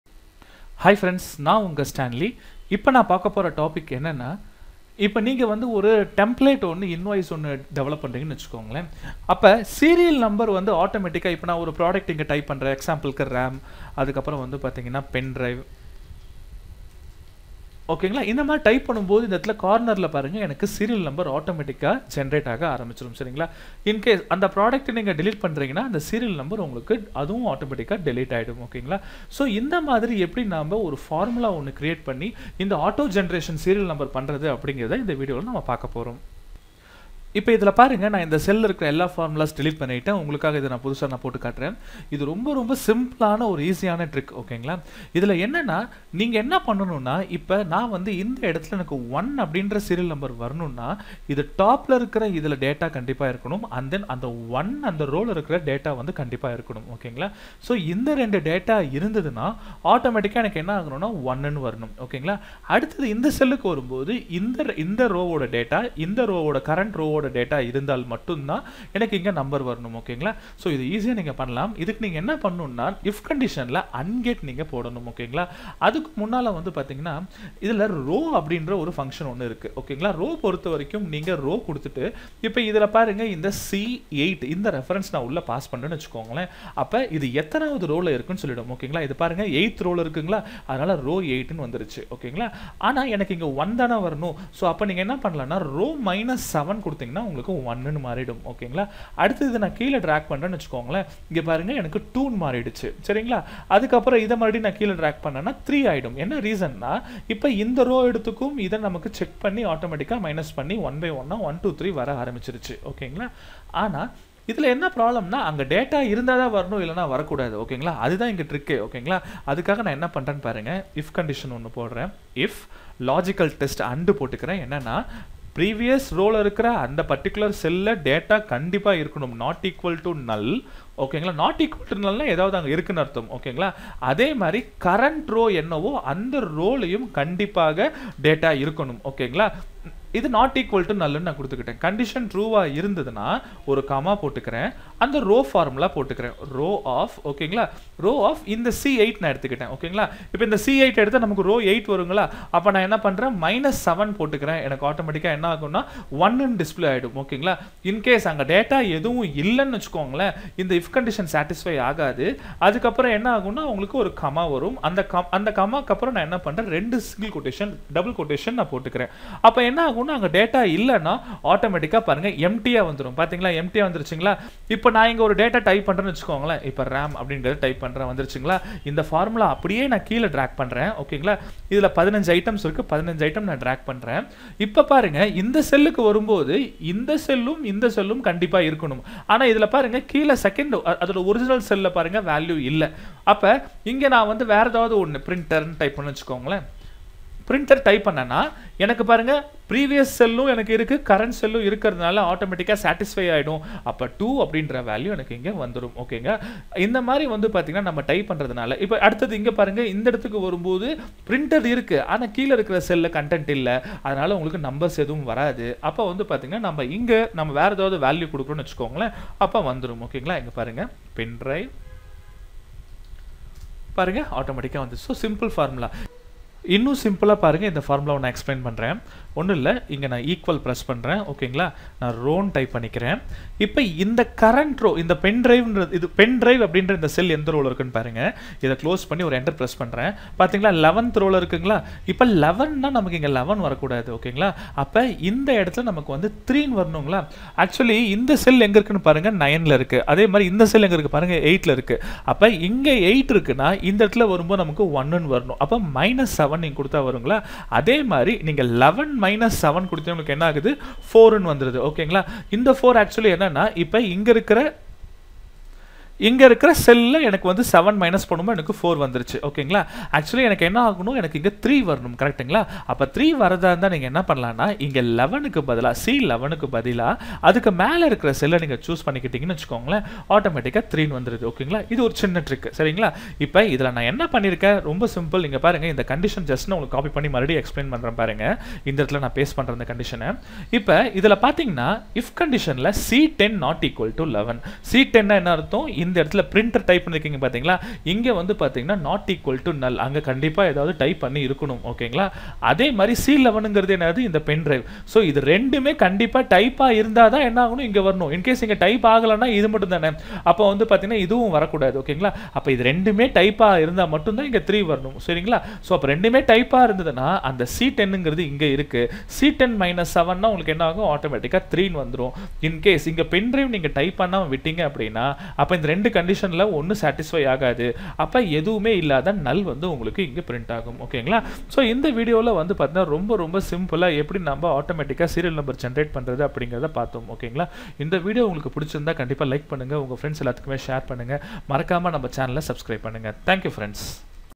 Automatically Serial Numbers in Microsoft Excel Tamil
How to Automatically Add Serial Numbers in Excel
Easy Ways to Number Rows in Excel